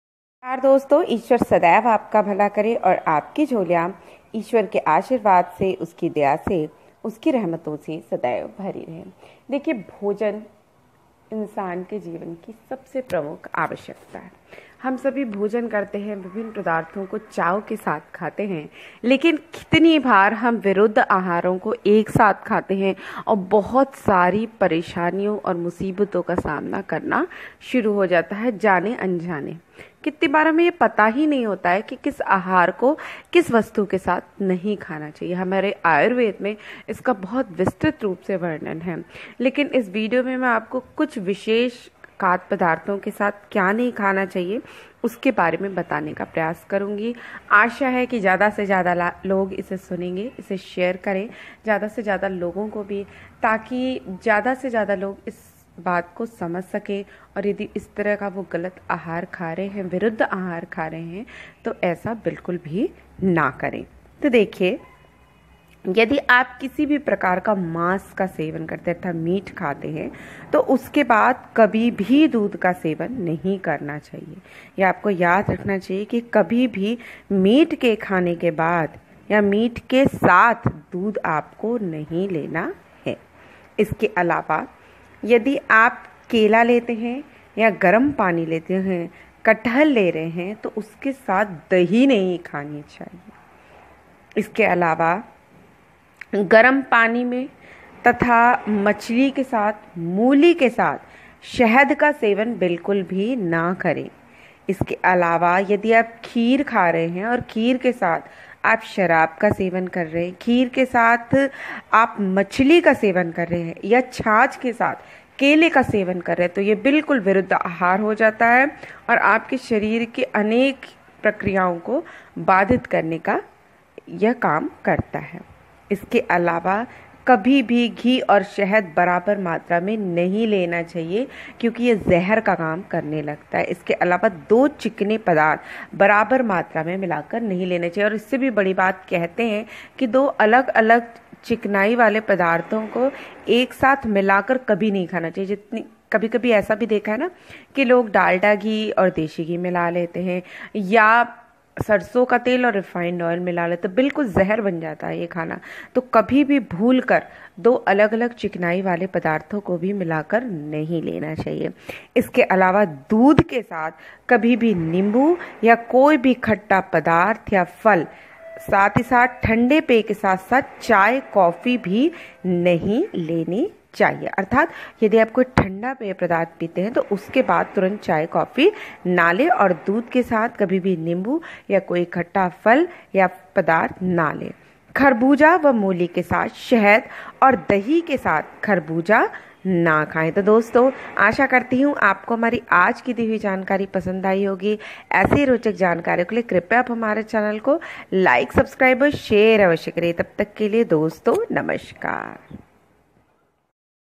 जानें किस खाने को किसके साथ नहीं खाना चाहिए kis khane ko kis cheej ke saath nahi khana chahiye.
Viruddh aahar aayurved ke anusaar.kis bhojan ko kiske saath naa khayen.